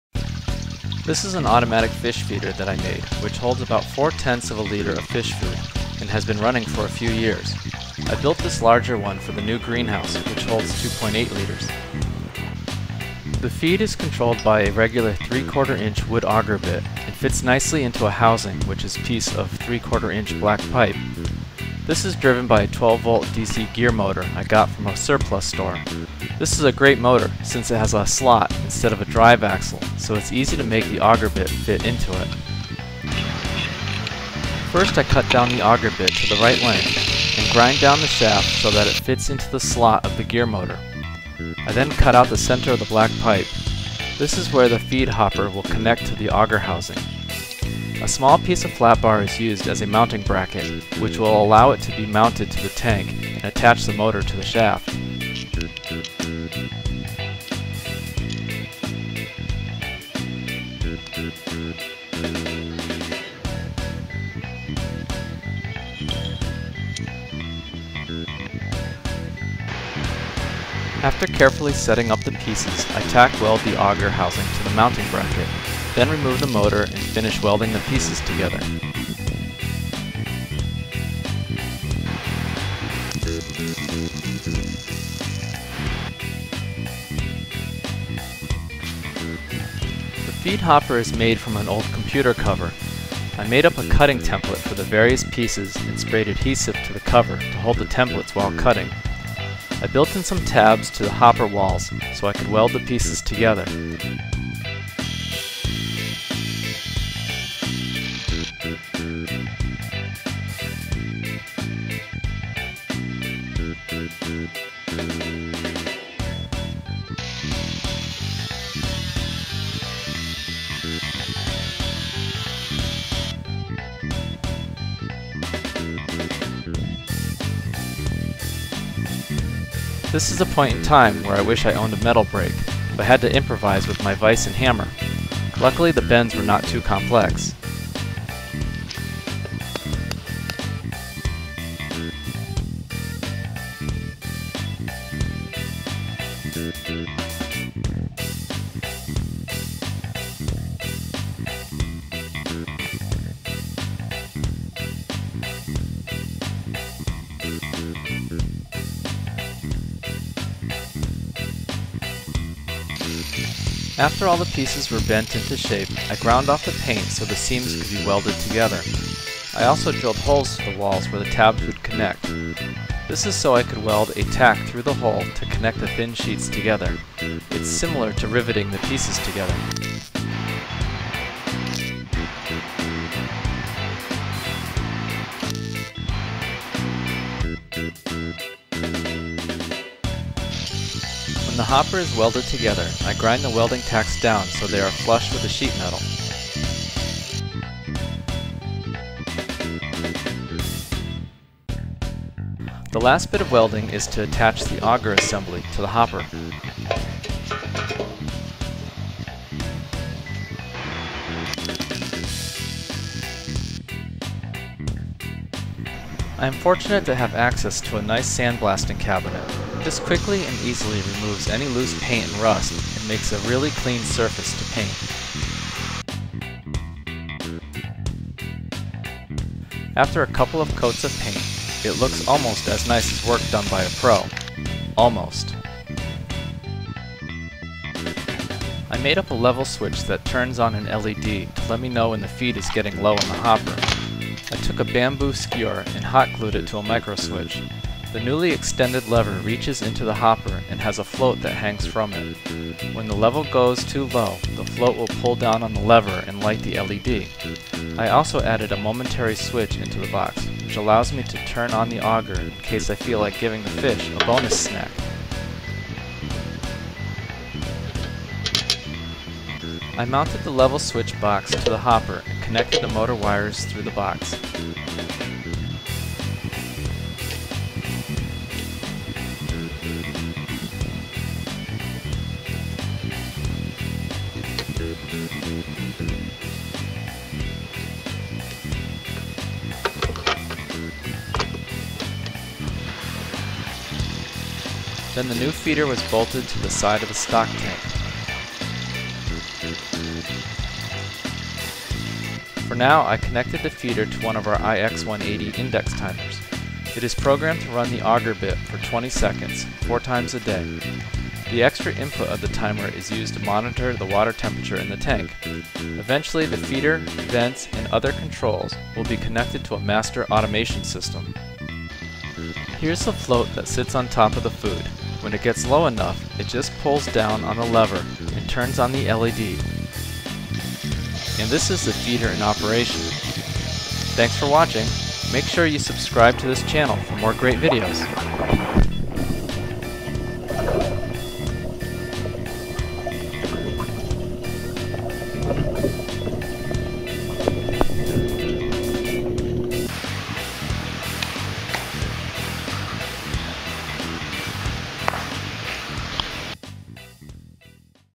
Automatic Fish Feeder
This is an automatic fish food dispenser I made for the new aquaponic system in our geodesic dome greenhouse.  It's set up to run on one of our IX series indexing timers.  For more information about the timer, please go